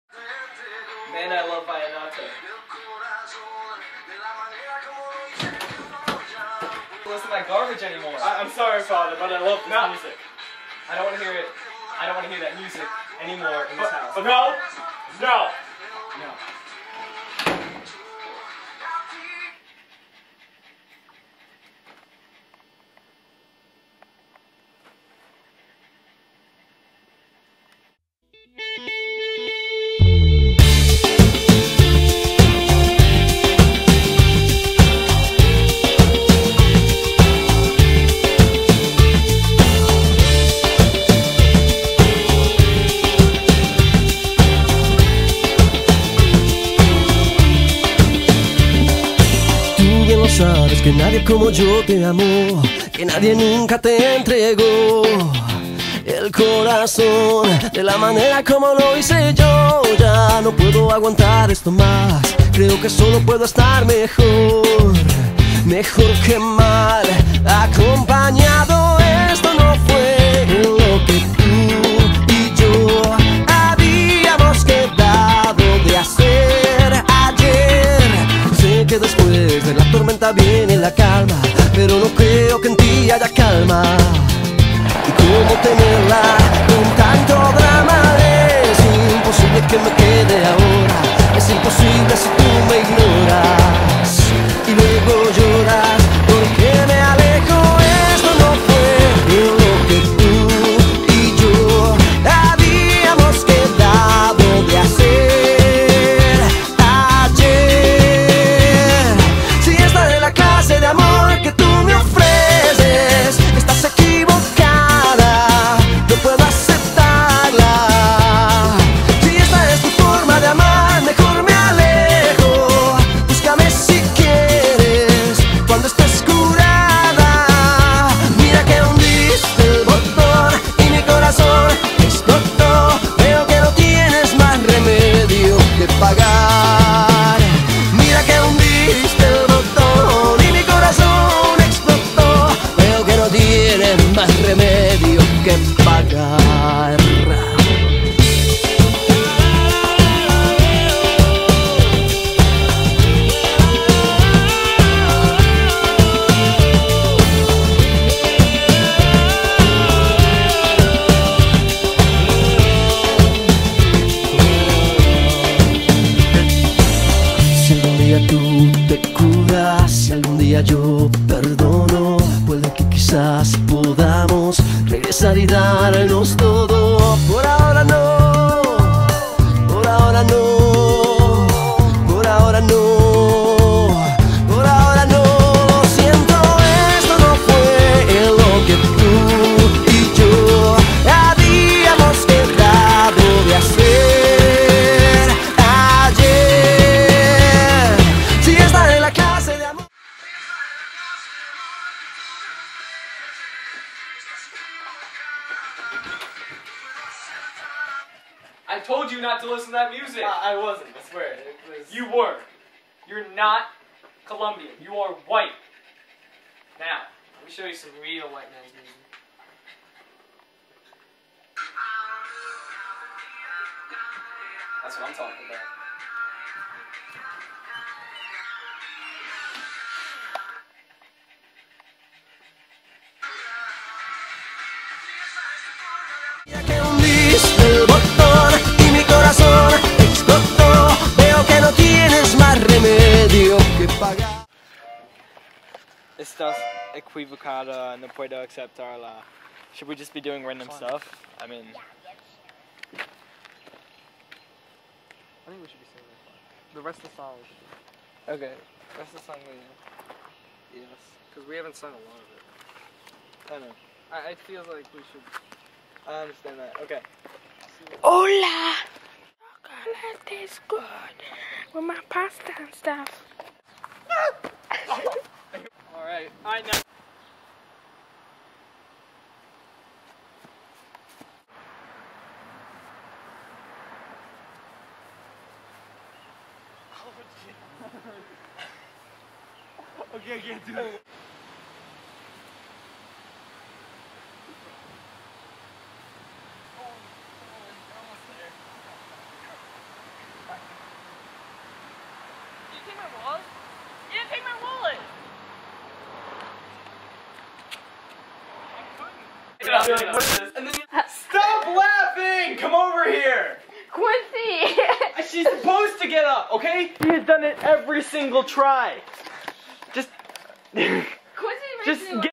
Vallenato Music Video
A music video we made for our spanish class.